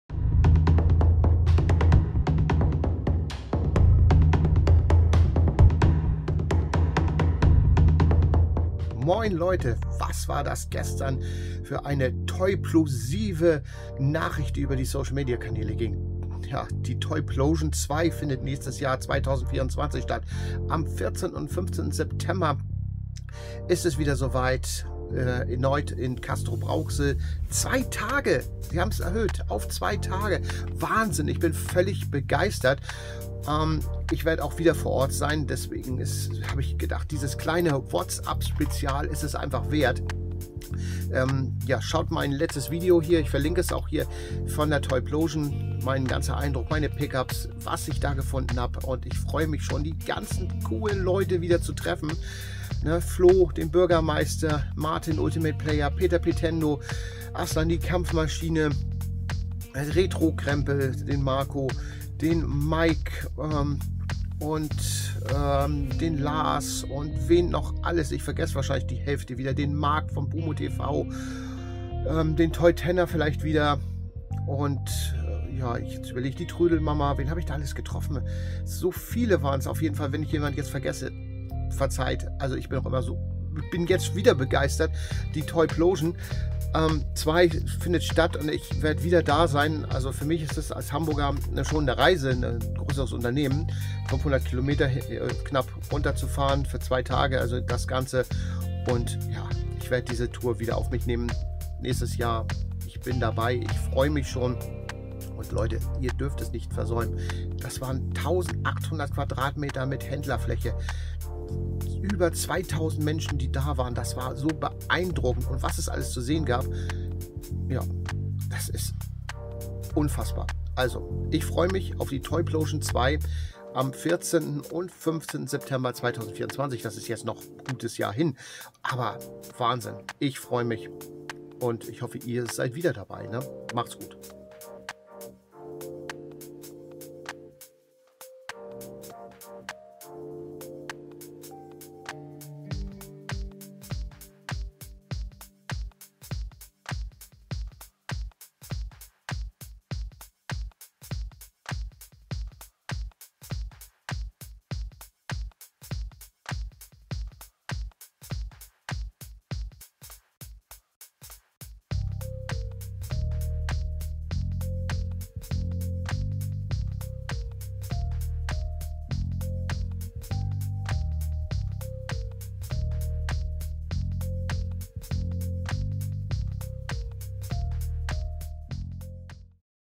2 kommt 2024 ... I'll be back! - What's Up Special - News, FAQ, Videos & Mehr
Die TOYPLOSION 2023 war der MEGA EVENT wenn es um Toys, Retro und sammeln geht. Nun die Sensation am 14. & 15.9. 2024 geht der toyplosive Event in die nächste Runde. Hier meine Ankündigung.
SHORTREVIEW RE-VIEW ist nicht nur Review, eine Betrachtung auch ein Rückblick.

Schaut auch in meine anderen Videos u.a. über TRANSFORMERS G1 rein.

SHORTREVIEW